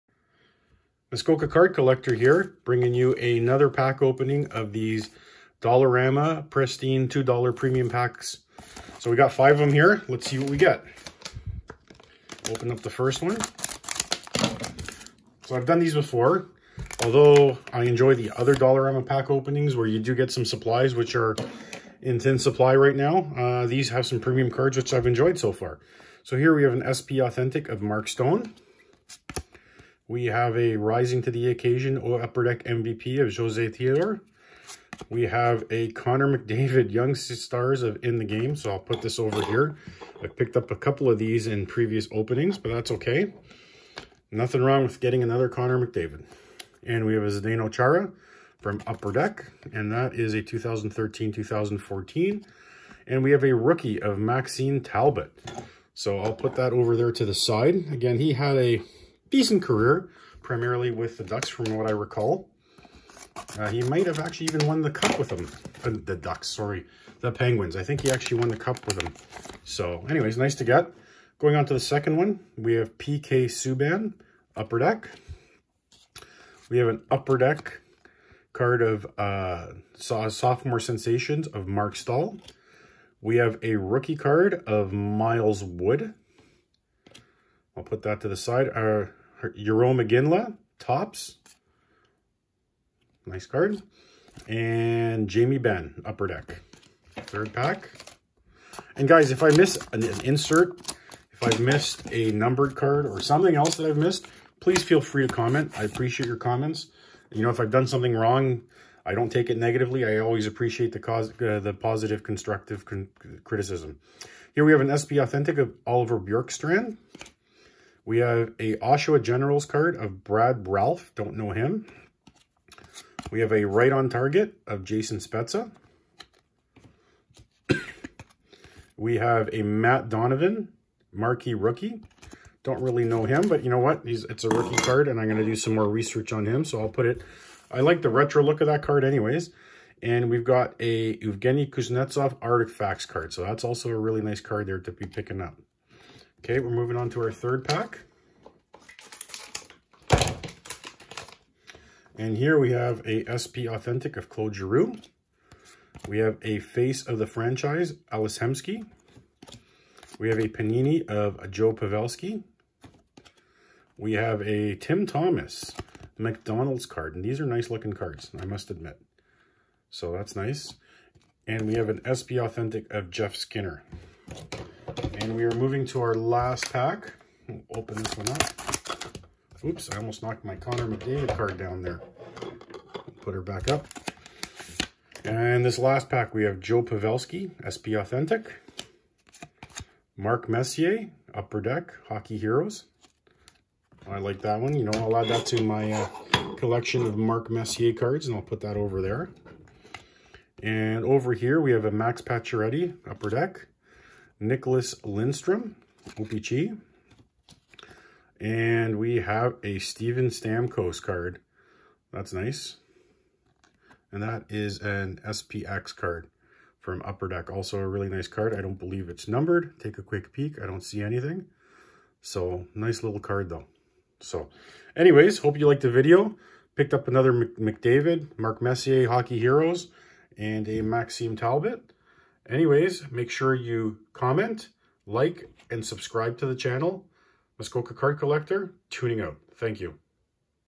Dollarama Presstine Premium Hockey Card Packs #3! - More Connor McDavid!
Opening up 5 Packs of Dollarama Presstine Premium Hockey Card Packs. These are supposed to contain Superstars, Hall of Famers, and Special Insert Cards - let's see what i get!  5 Cards per Pack